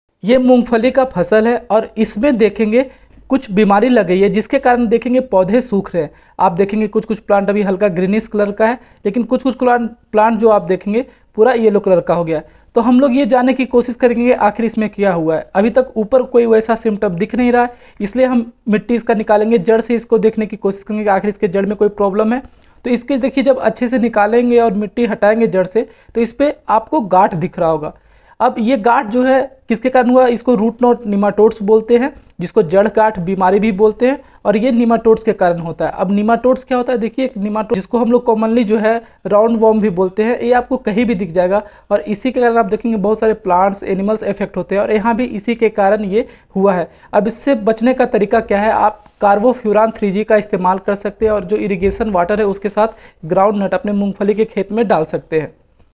मूंगफली में जड़ गाठ || Root knot disease in Groundnut
in this video you will know about root knot disease caused by nematodes